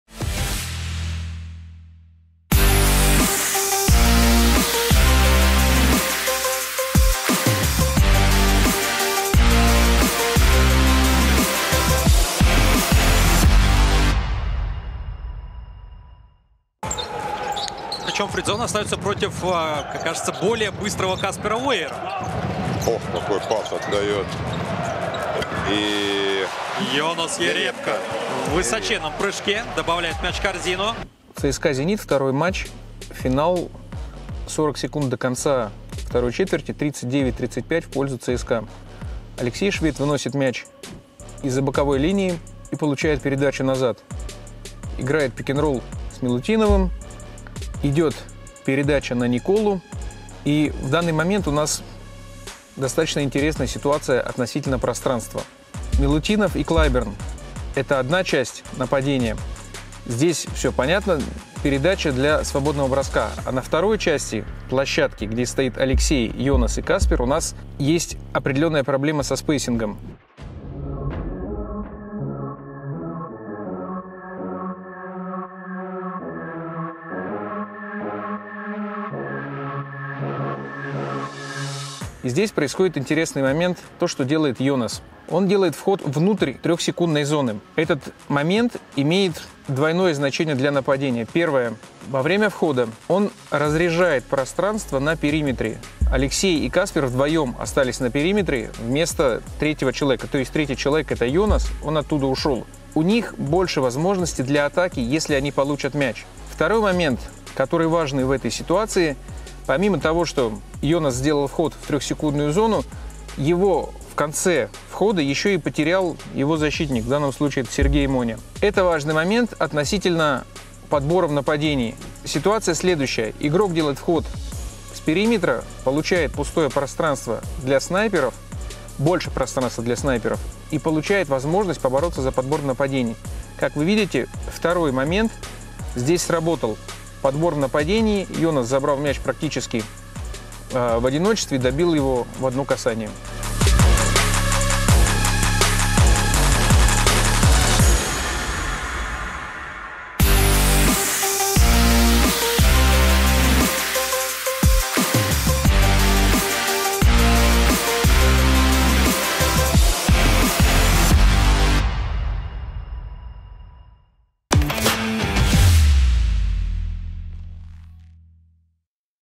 «Разбор». Добивание в одном прыжке Йонаса Йеребко во втором матче финальной серии
Скаут ЦСКА Денис Годлевский разбирает момент из второго матча финальной серии Единой лиги ВТБ против «Зенита»